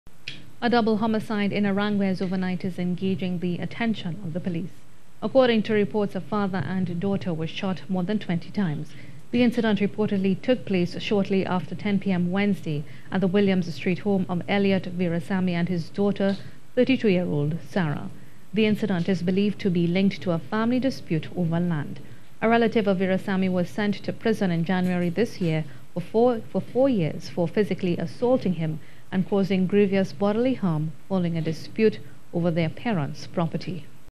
Double Murder In Aranguez - Father And Daughter Killed In Suspected Land Dispute
A father and daughter were shot more than 20 times in a double homicide in Aranguez.

The incident reportedly took place shortly after 10pm Wednesday at the Williams Street home of Elliot Verasammy and his 32 year old daughter, Sarah. The incident is believed to be linked to a family dispute over land.

A relative of Verasammy was sent to prison in January this year for four years for physically assaulting him and causing grievous bodily harm following a dispute over their parents' property.